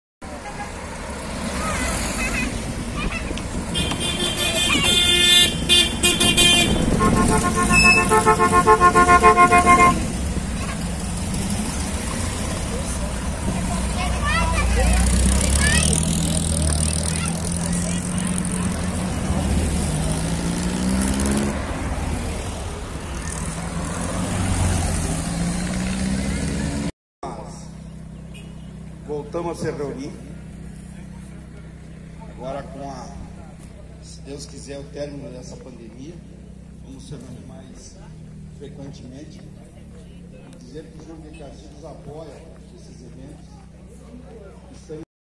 Associação Amigos do Fusca - Santa Maria/RS
Associação Amigos do Fusca - Santa Maria/RS  se fez presente na cidade de Júlio de Castilhos, na carreata em comemoração dos 130 anos da cidade